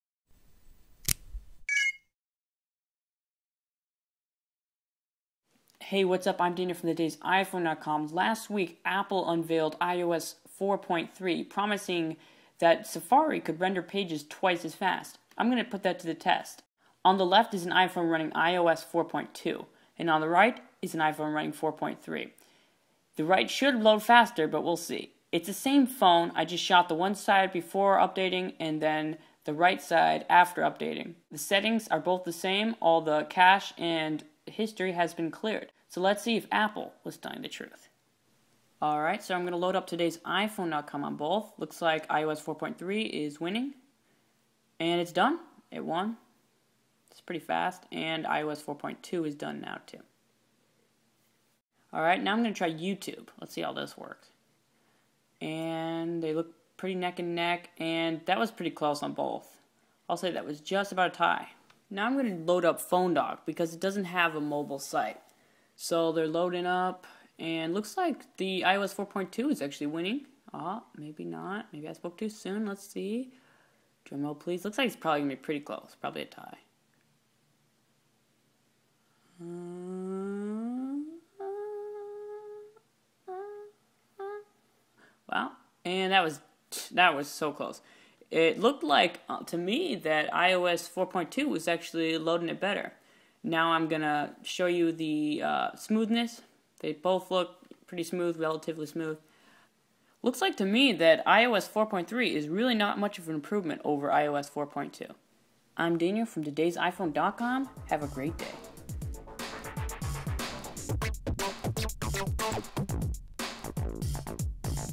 iPhone 4 with iOS 4.3: Safari Speed Test
iOS 4.3 Speed test: Apple made a big deal about Safari optimizations in the new software update. But is browsing really any faster than iOS 4.2? Daniel checks it out on the iPhone 4.